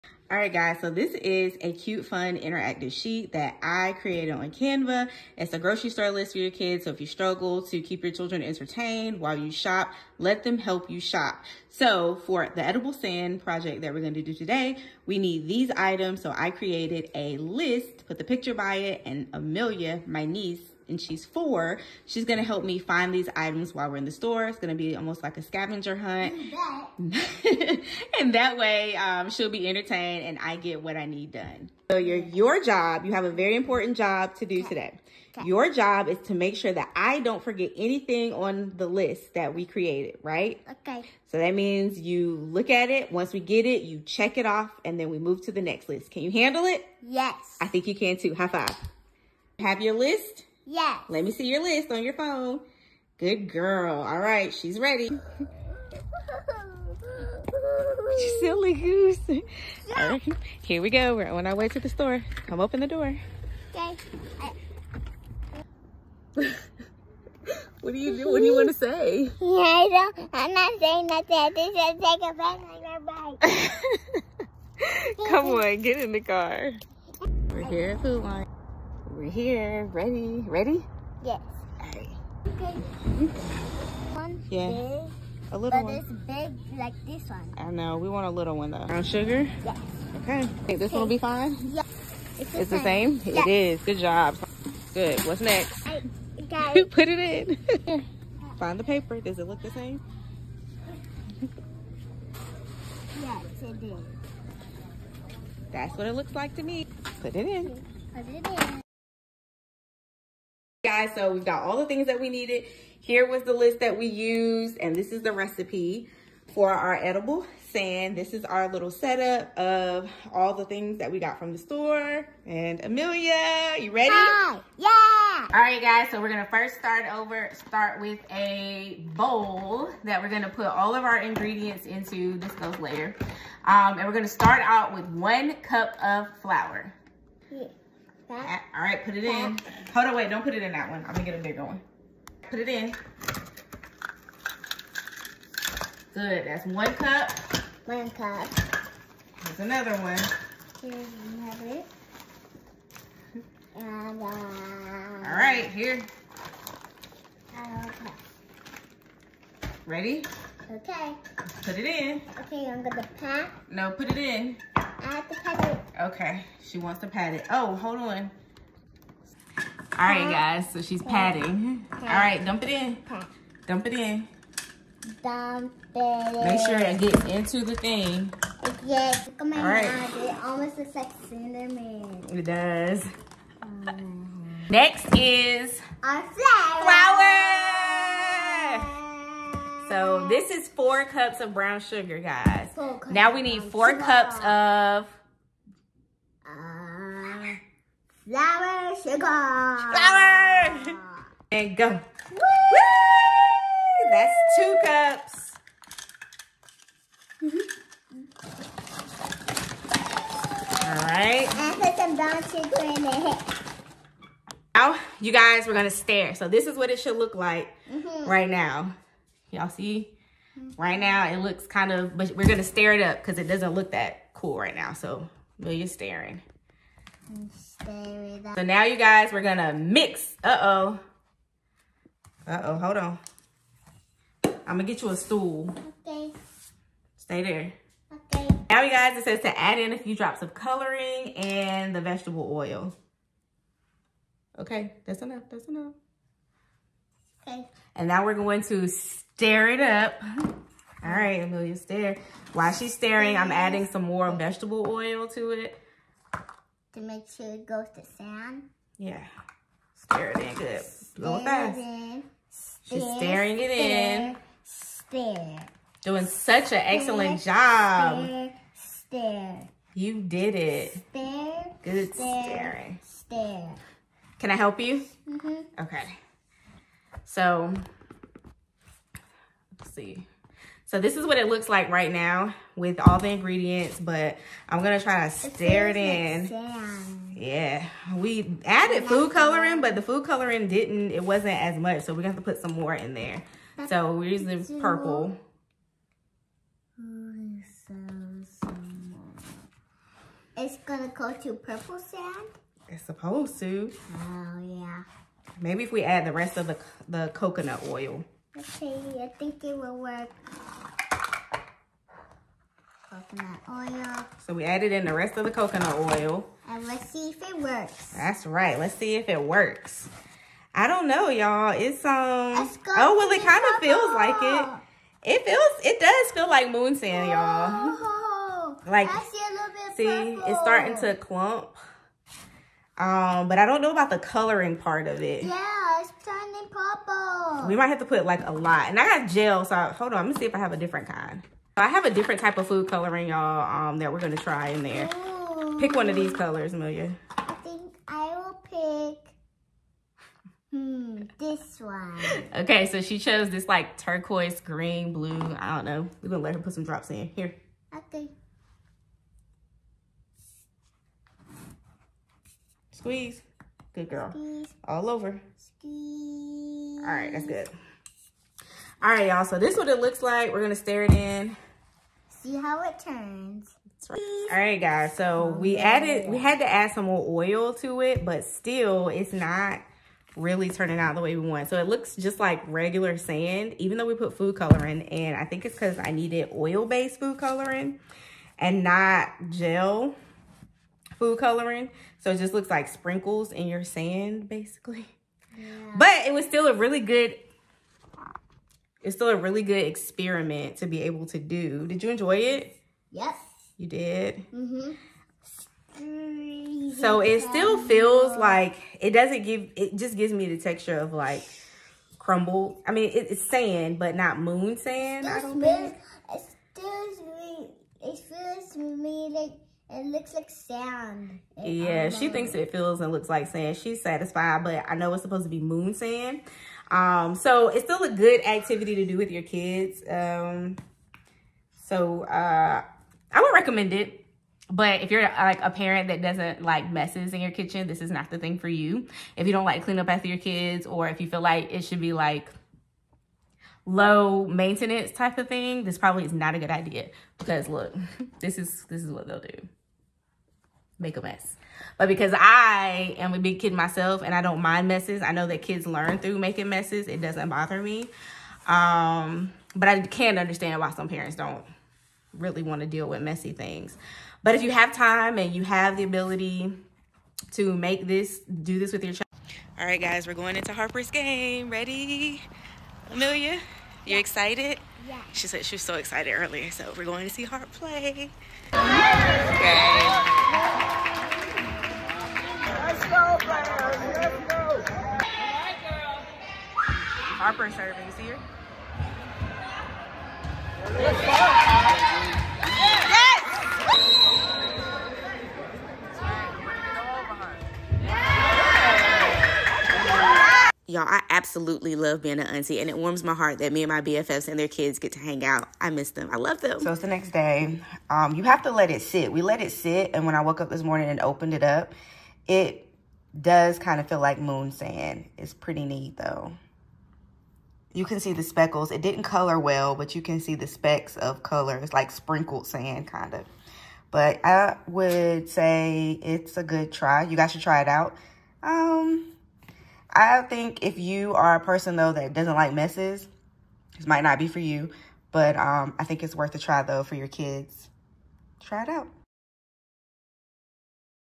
Auntie and Niece Day: Ultimate Edible Moon Sand
Auntie and Niece Day! We made the ultimate Edible moon sand. We also went grocery shopping and attended a volleyball game.

00:00 Introduction
01:19 Grocery Store Shopping
02:16 Making Moon Sand
07:53 Volleyball Game